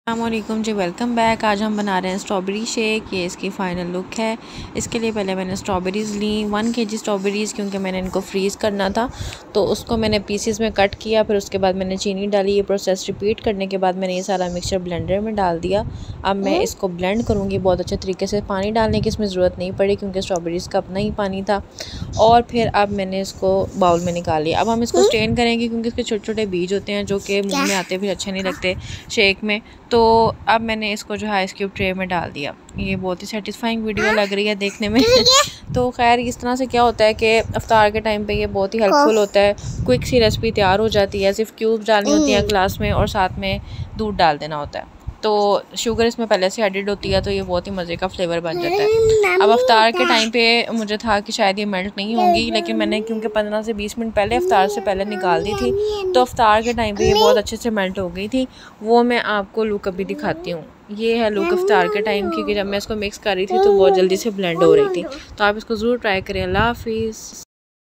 Strawberry Shake|How to Freeze Strawberry|Iftaar time|Refreshing Drink|Ramazan Series
Cut 1kg strawberry, then put it in a bowl, add sugar.
Blend this mixture well, then pour it into icecube tray
freeze
enjoy in iftaar :)
 for shake:
add 3-4 cubes in a glass of milk, stirr, enjoy
for juice:
add 3-4 cubes in a glass of water, stirr, enjoy